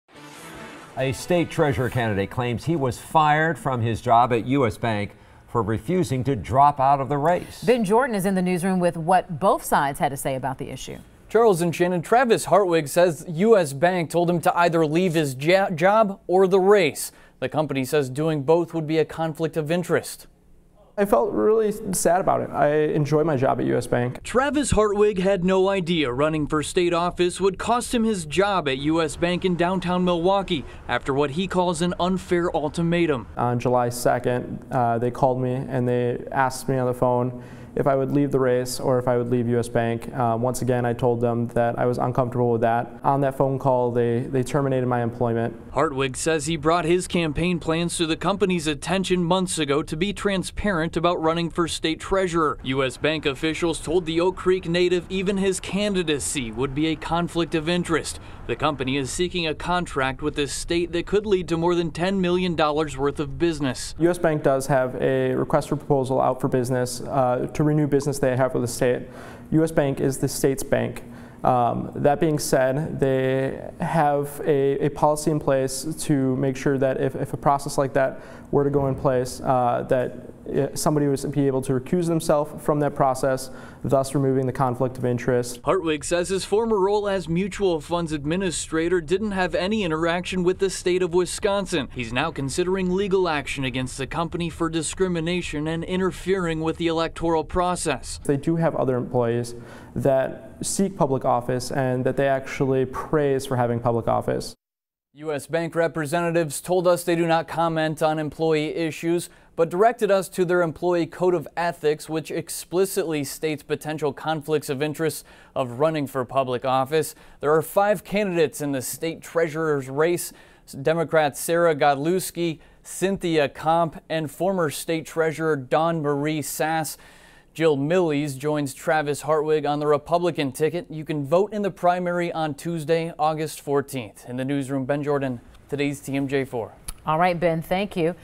State treasurer candidate fired from job for refusing to drop out of race
Travis Hartwig said the company told him to either leave the race or his job. U.S. Bank said doing both would be a conflict of interest.